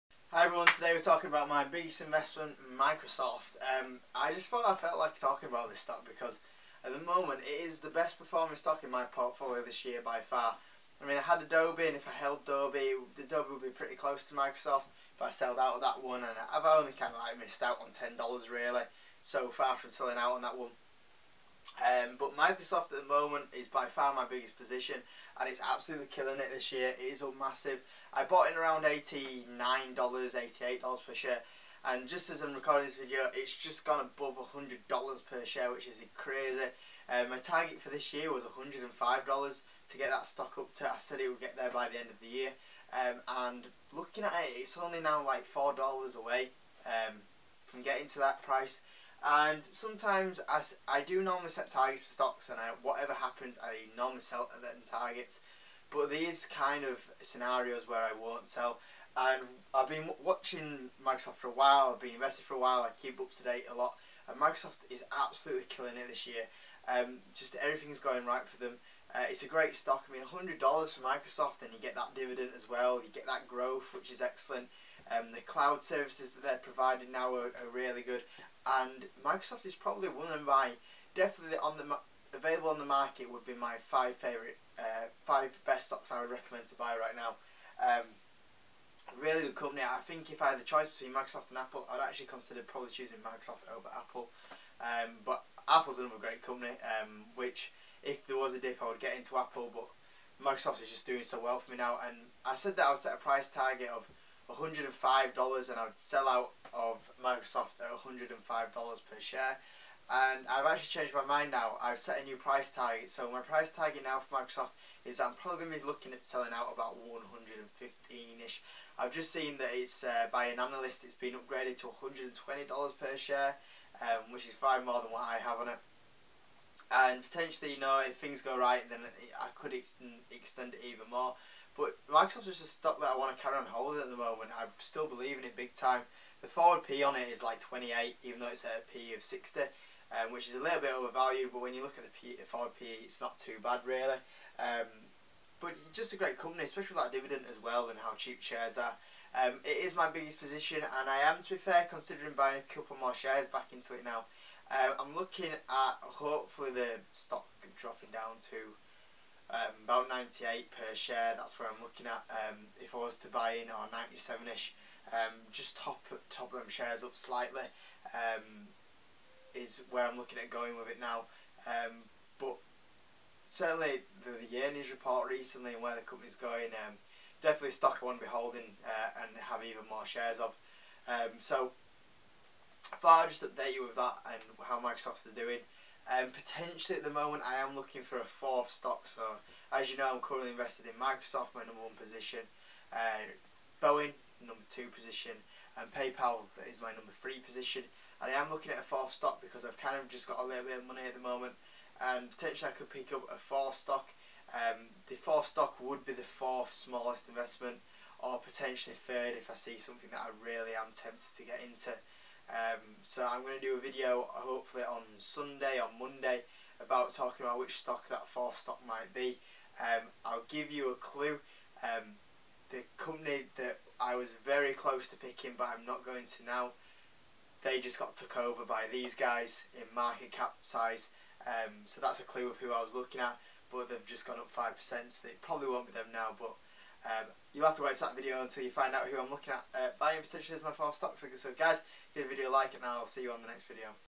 Microsoft Stock Is Crazy!
Hi Everyone just updating you on my position in Microsoft